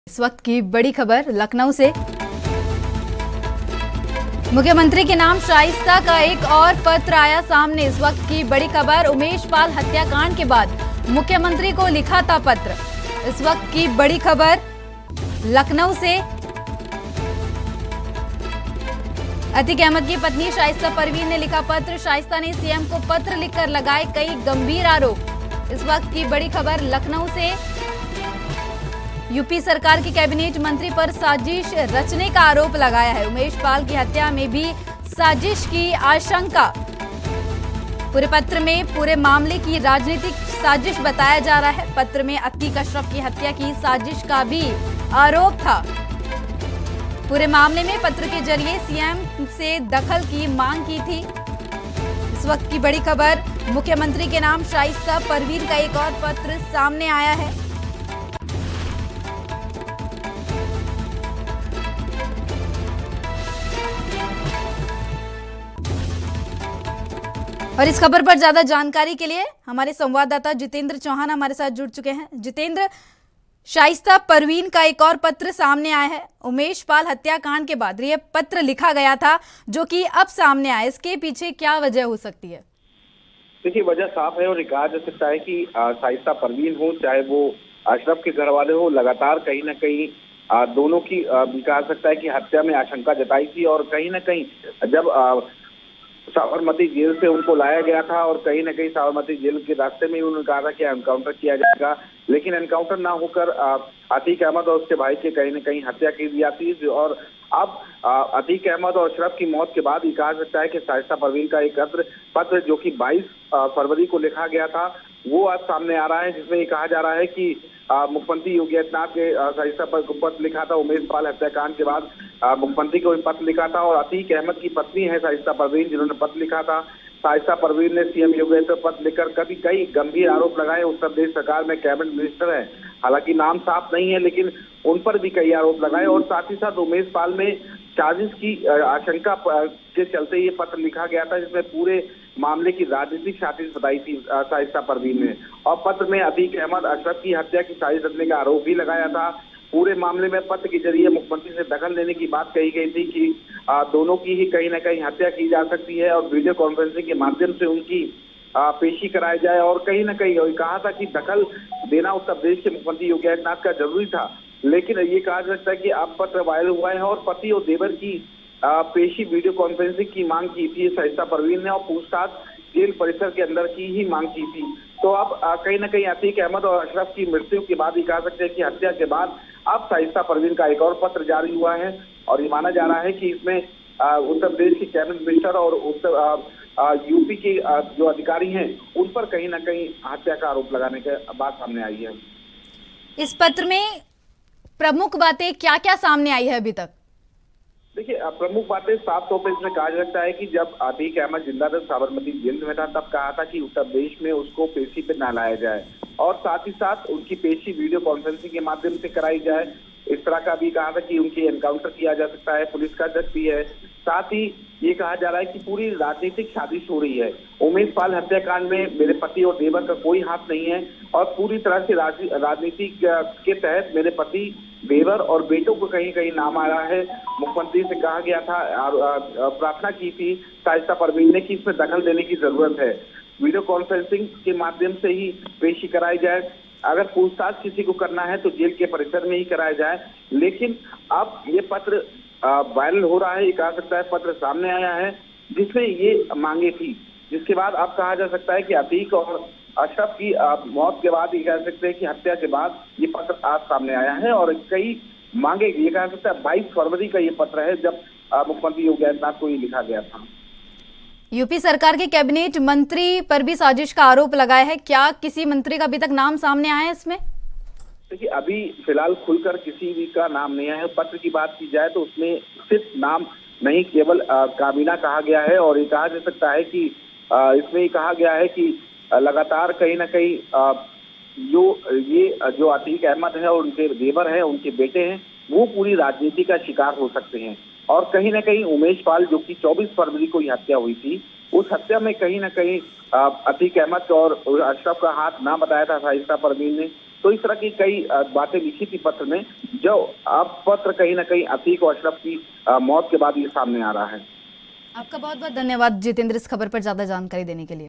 यूपी: मुख्यमंत्री के नाम शाइस्ता का एक और पत्र आया सामने | ANB NEWS
यूपी: मुख्यमंत्री के नाम शाइस्ता का एक और पत्र आया सामने
उमेश पाल हत्याकांड के बाद मुख्यमंत्री को लिखा था पत्र
अतीक अहमद की पत्नी शाइस्ता परवीन ने लिखा था पत्र
शाइस्ता ने सीएम को पत्र लिखकर लगाए कई गंभीर आरोप
यूपी सरकार के कैबिनेट मंत्री पर साजिश रचने का आरोप
उमेश पाल की हत्या में भी साजिश की आशंका
पत्र में पूरे मामले को राजनीतिक साजिश बताया
पत्र में अतीक -अशरफ की हत्या की साजिश का भी आरोप था
पूरे मामले में पत्र के जरिए CM से दखल की मांग की थी